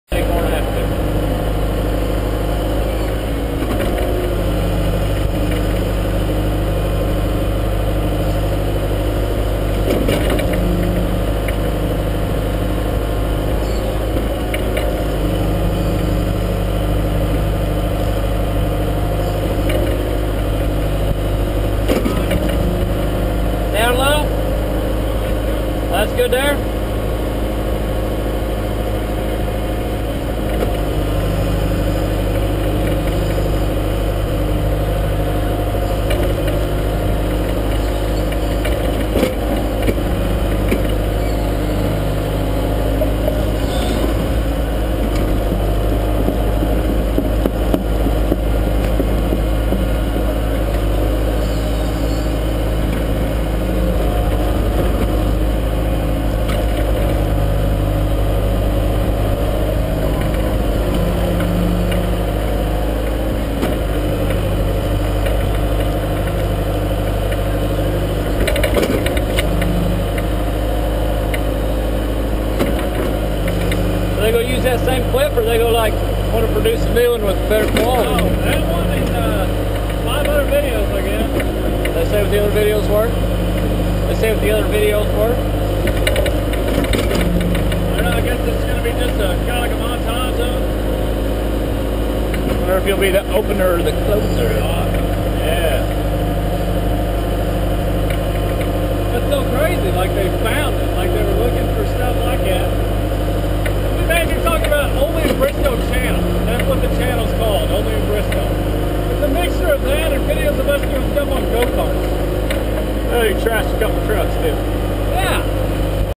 Trenching with a John Deere 120 excavator and a custom bucket
using our tranching knife